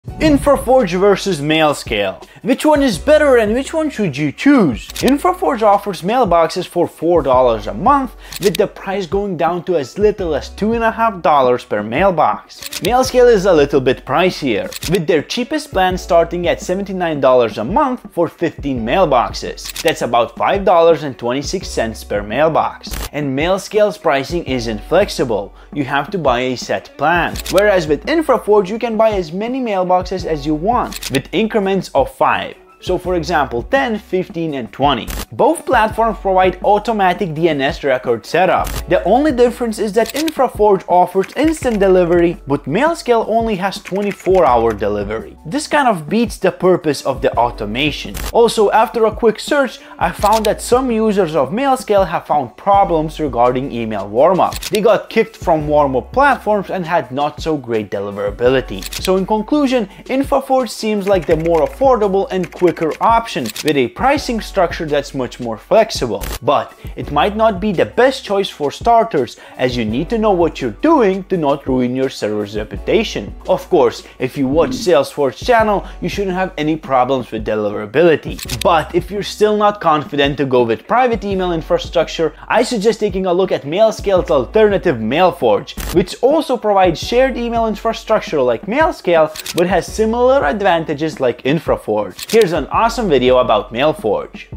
Infraforge VS Mailscale (Private VS Dedicated IP for email infrastructure)
In this comparison video, we'll break down the differences between Infraforge and Mailscale for your email infrastructure needs. Whether you're looking for a private or dedicated IP solution, we'll help you decide which option is best for your business.